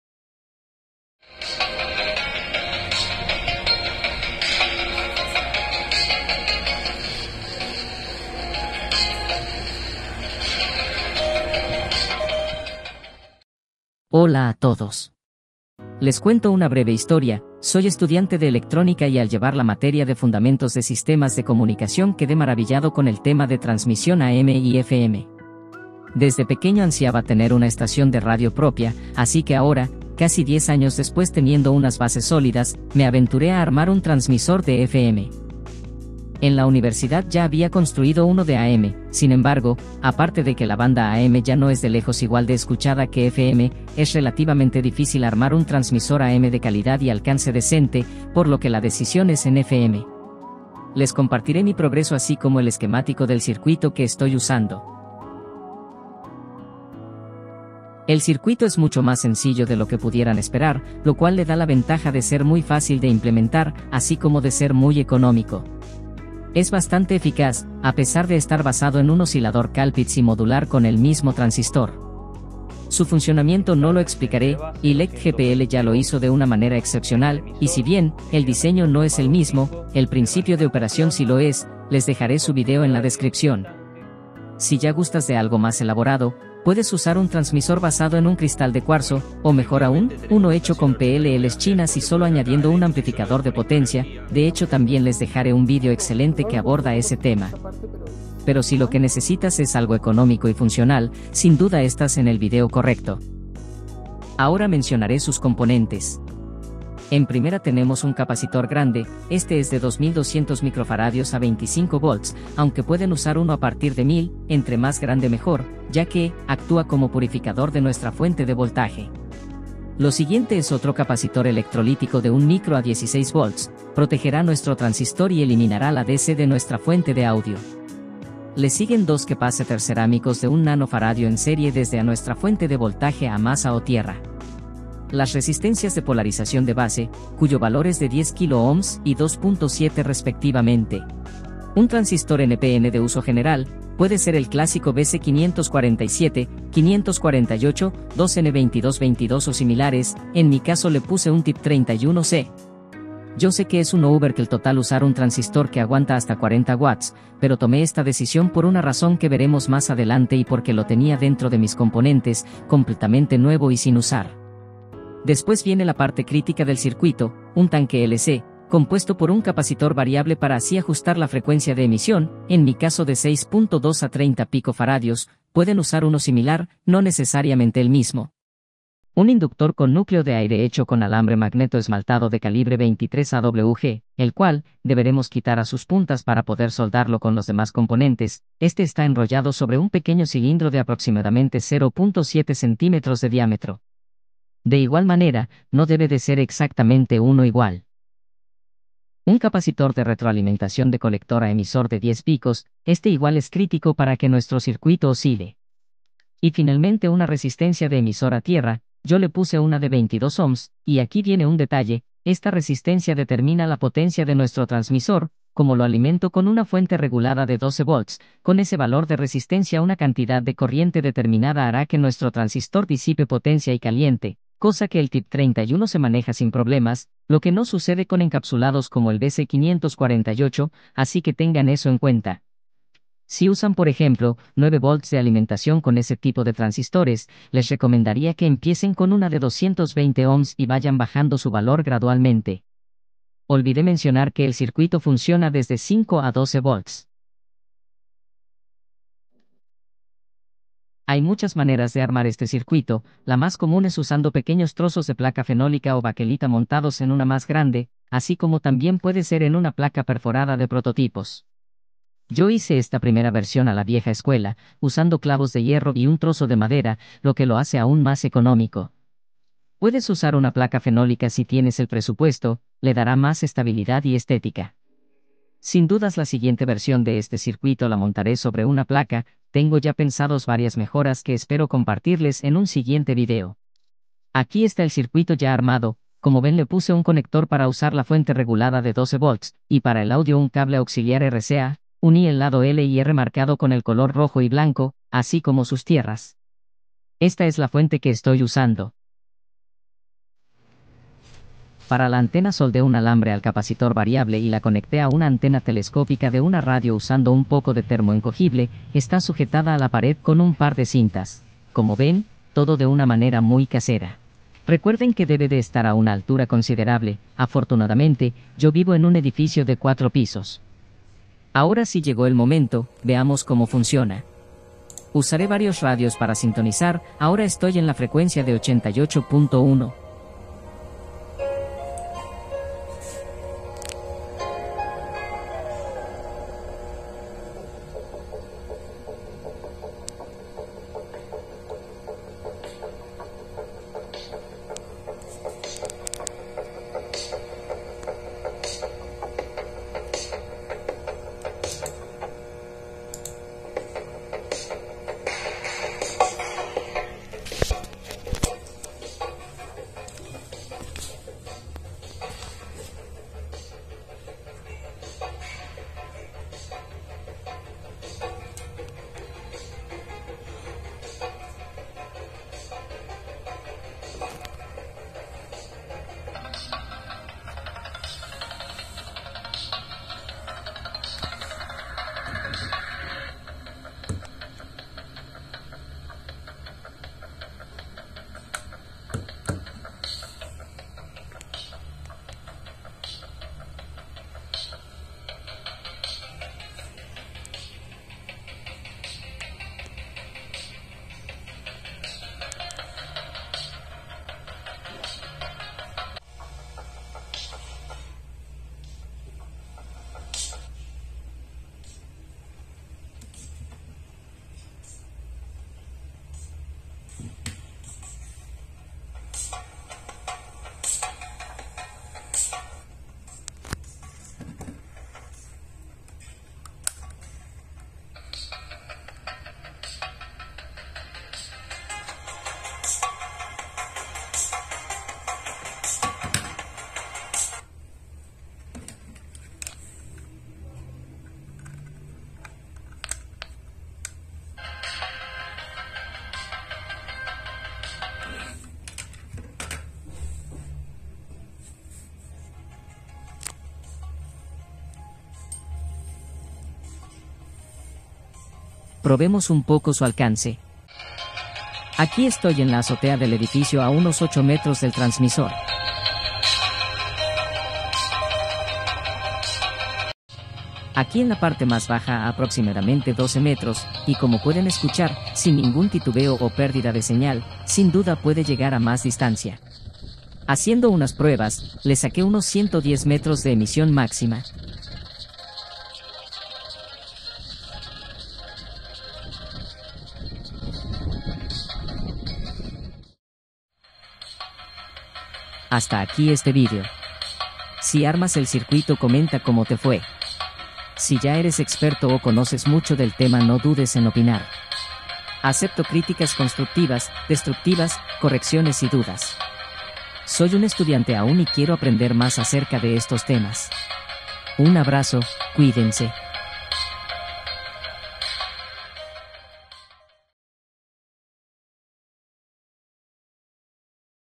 HAZ TU TRANSMISOR DE RADIO FM CASERO, FÁCIL Y ECONÓMICO (BUENA DISTANCIA)
Hola amigos. Espero que les haya gustado el vídeo, deja tu opinión o comentario.

Nota: Puedes ampliar su alcance con una mejor antena.